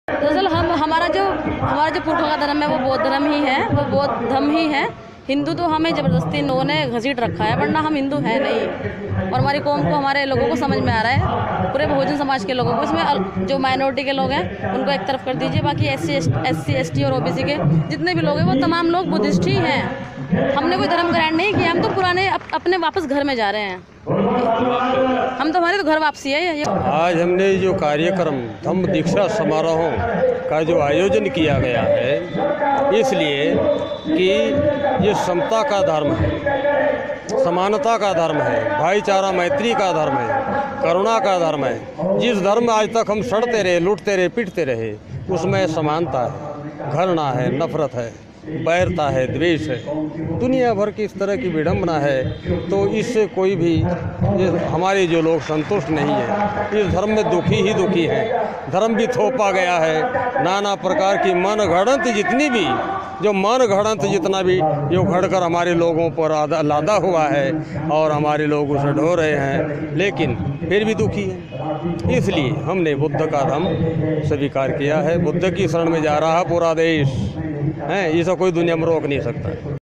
सहारनपुर में सैंकडो़ं लोगों ने धर्म परिवर्तन कर अपनाया बौद्ध धर्म, बोले- अब हुई है घर वापसी
सहारनपुर: भारतीय बौद्ध महासभा के बैनर तले बौद्ध धर्म सम्मेलन के कार्यक्रम में सैंकड़ों लोगों ने बौद्ध धर्म अपना लिया. इसमें महिलाओं सहित कई श्रद्धालु मौजूद रहे. बौद्ध धर्म स्वीकार करने के बाद लोगों ने बताया कि वह अब भगवान बुद्ध की शरण में रहकर आपसी भाईचारे और विश्व शांति की बात को रखेंगे.